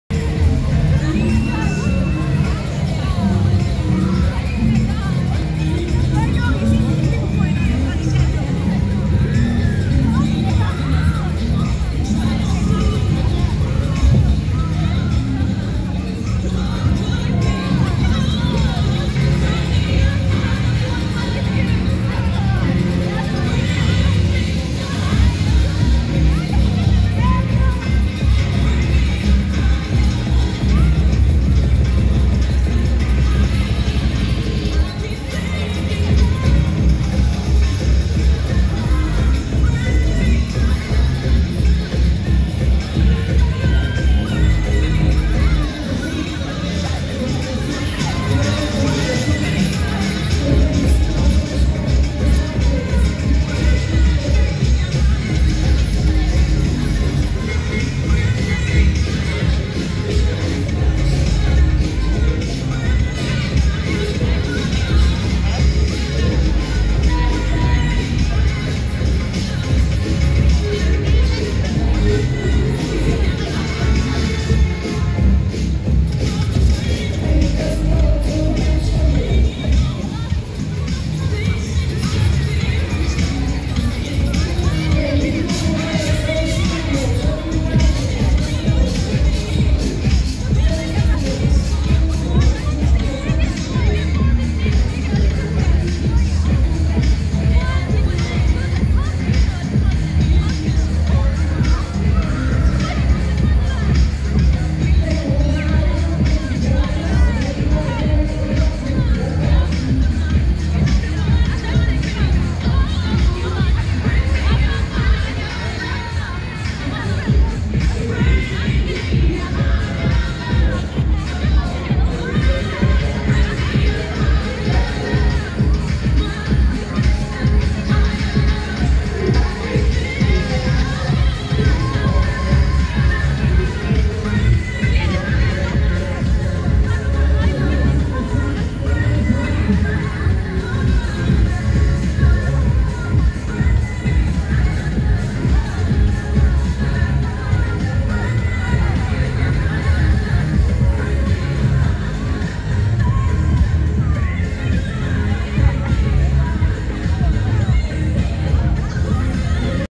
Leigh danter funfair ride take off 40 meters @ cardiff calennig fair 2019
leigh danters starflyer funfair ride video filmed cardiff calennig fair.